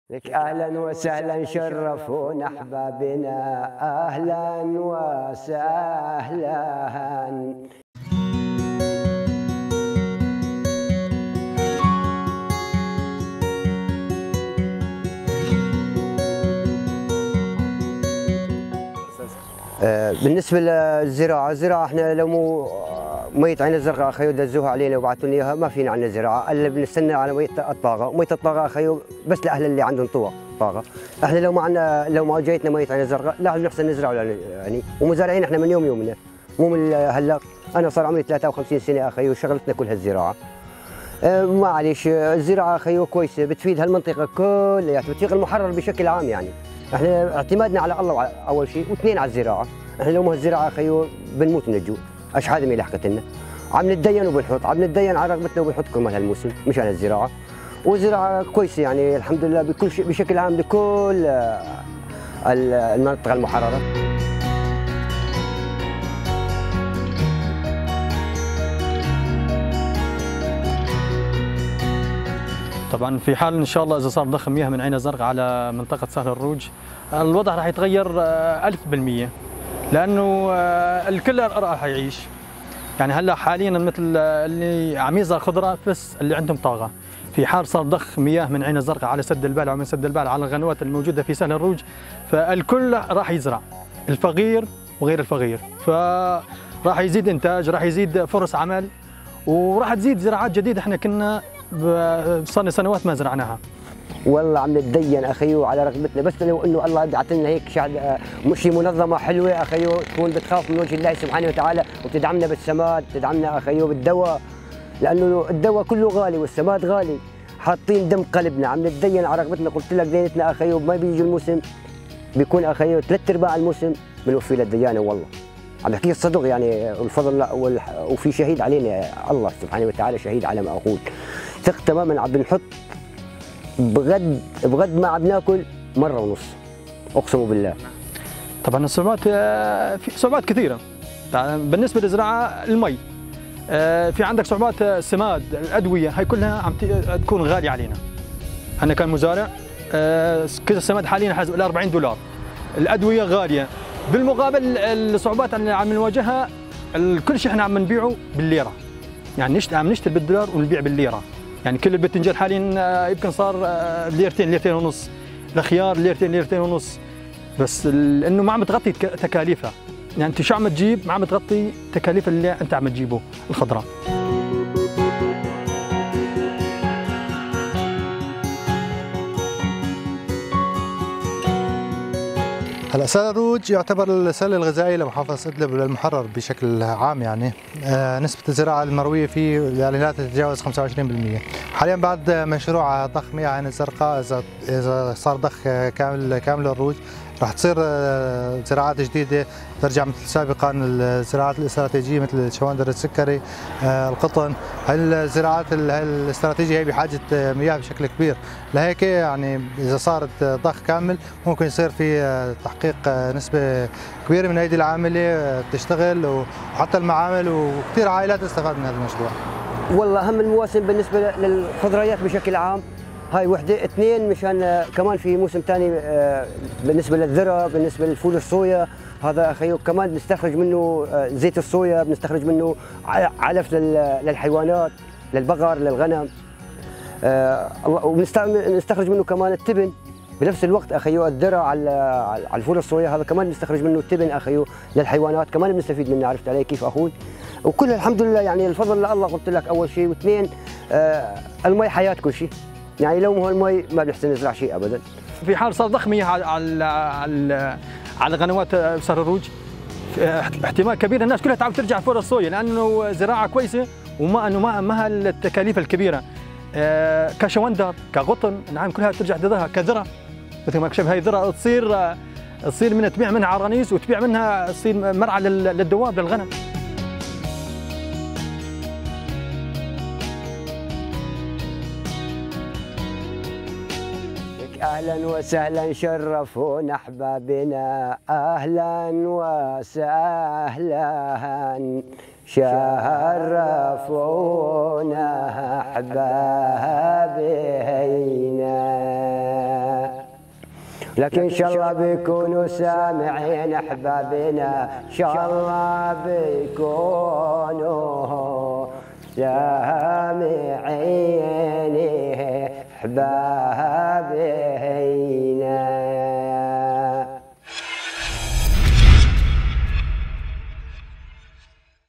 "بدون الزّراعة منموت من الجوع".. مياه "عين الزرقاء" تروي أراضي سهل الروج وتُحيي آمال المزارعين
ضخُّ المياه من محطة عين_الزرقاء يُحيي آمال المزارعين لتنشيط زراعتهم النوعية والإستراتيجية في سهل_الروج بريف